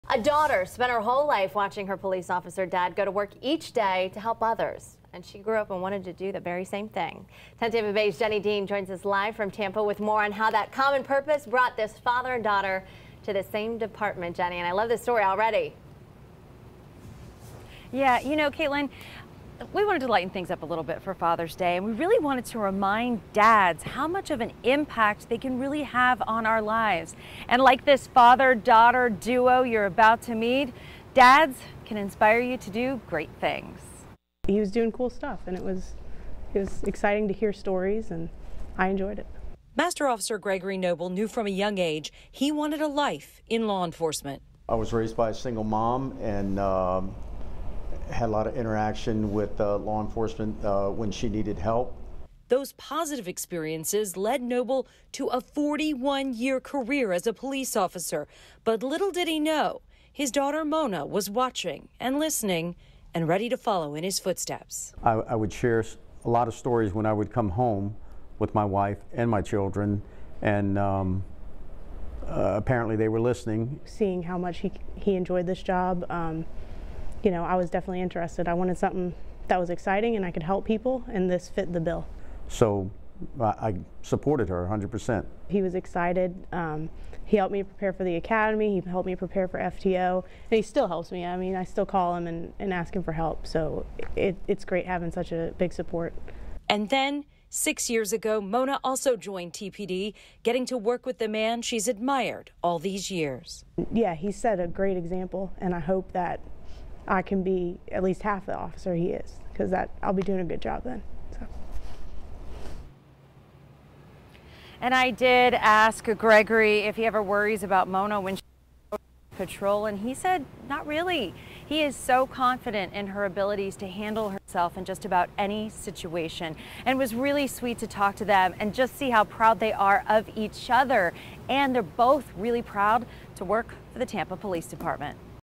Tampa police officer following in dad's footsteps
Growing up watching her dad and listening to his stories, Mona knew she wanted to be just like him.